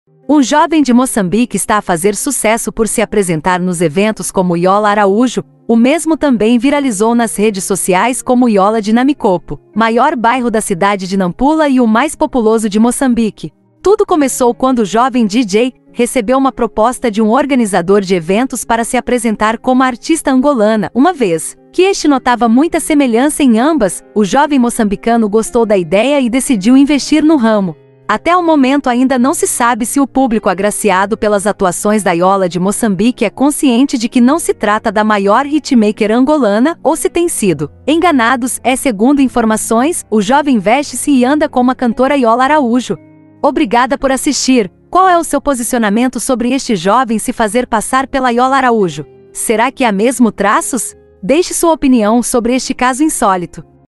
CASO INSÓLITO : JOVEM APRESENTA-SE COMO YOLA ARAÚJO MOÇAMBICANO '' SOMOS GÊMIAS ''
Um jovem de Moçambique está a fazer sucesso por se apresentar nos eventos como Yola Araújo, O mesmo também viralizou nas redes sociais como Yola de Namicopo (maior bairro da cidade de Nampula e o mais populoso de Moçambique),

Tudo começou quando o jovem Dj, recebeu uma proposta de um organizador de eventos para se apresentar como a artista angolana, uma vez, que este notava muita semelhança em ambas,O jovem moçambicano gostou da ideia e decidiu investir no ramo,

Até ao momento ainda não se sabe se o público agraciado pelas atuações da Yola de Moçambique é consciente de que não se trata da maior hitmaker angolana ou se têm sido enganados,s egundo informações, o jovem veste-se e anda como a cantora Yola Araújo,